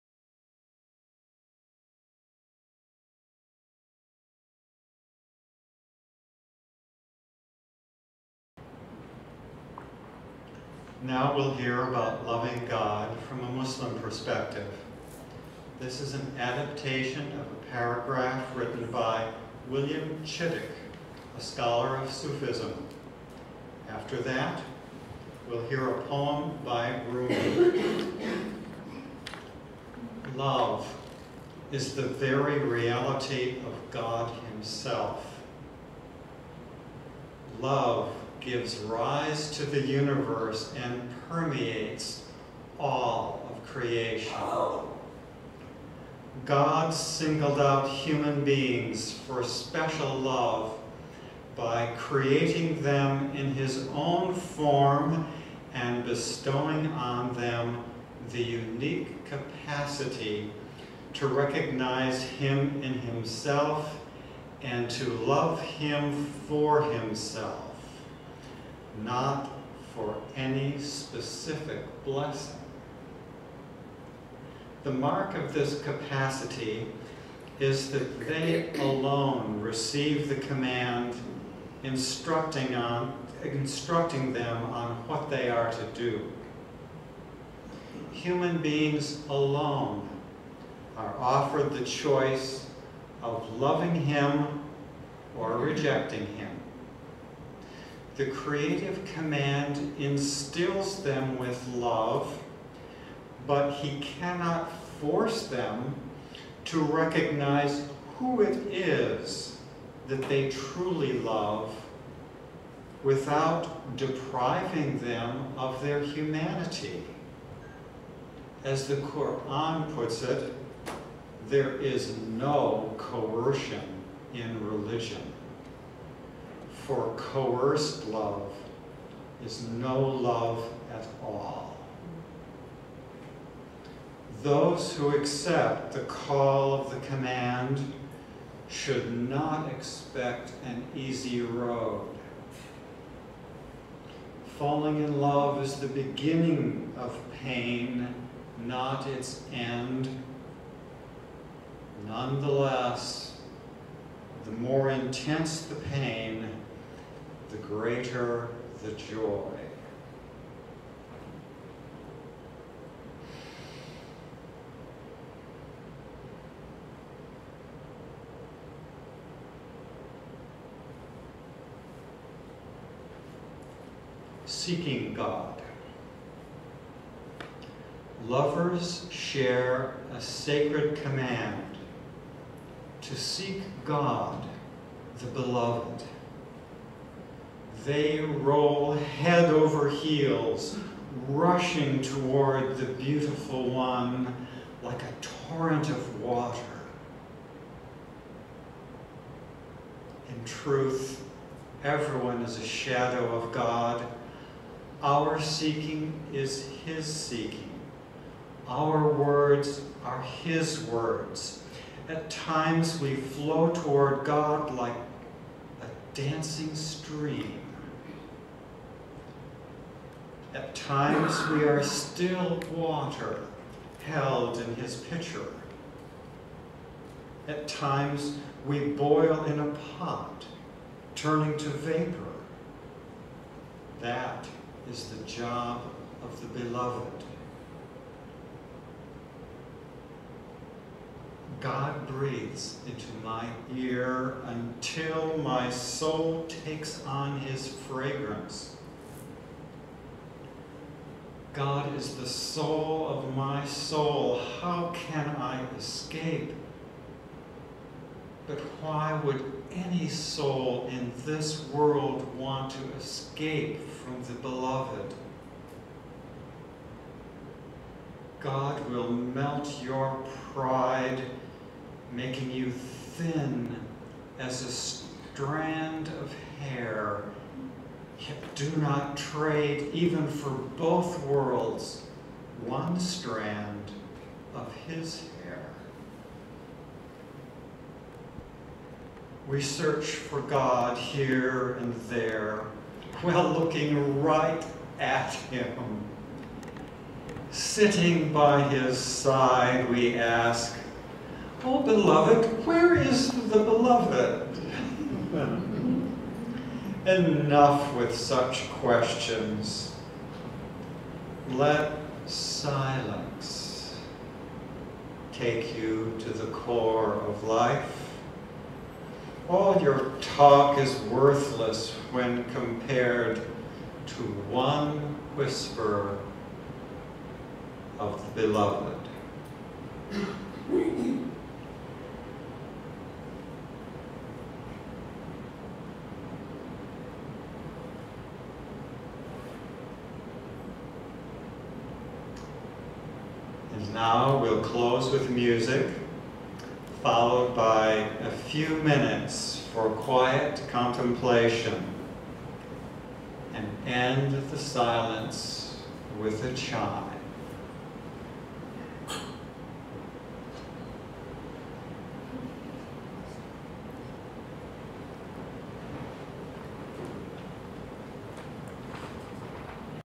Urantia Fellowship Summer Study - Jeff Wattles
Jeff Wattles provides meditations on the Muslim perspective on love at the Urantia Fellowship Summer Study Session, 2015